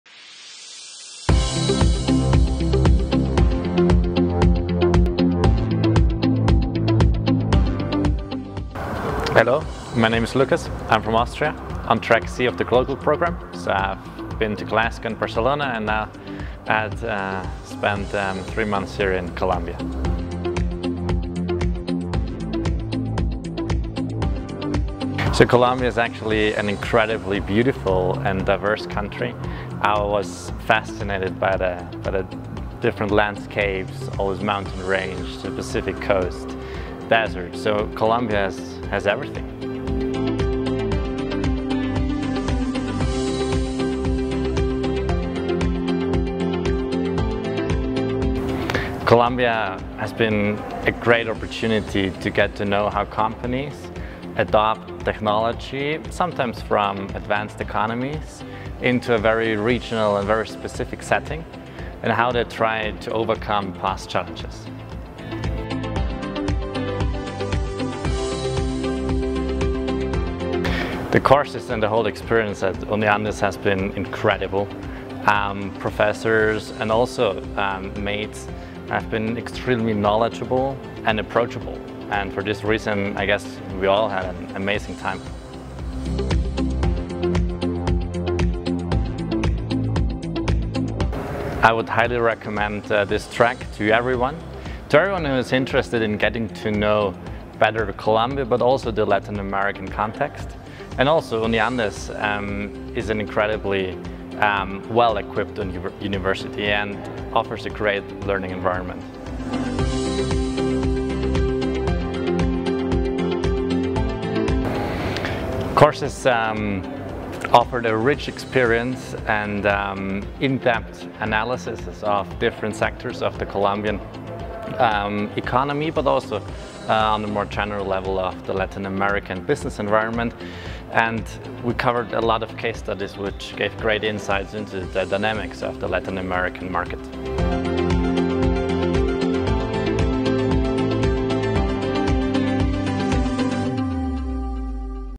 Glocal 2021 | Lukas Tanzmayr
En octubre de 2021, la Facultad de Administración le dio la bienvenida al primer grupo de seis estudiantes que estuvo en la Universidad de los Andes por un periodo de tres meses como parte de GLOCAL, un programa que está orientado hacia quienes deseen perfilarse profesional y académicamente, en organizaciones internacionales de política pública y urbana, en empresas multinacionales, y de consultoría, industrias creativas u organizaciones no gubernamentales. Así fue la experiencia de Lukas Tanzmayr.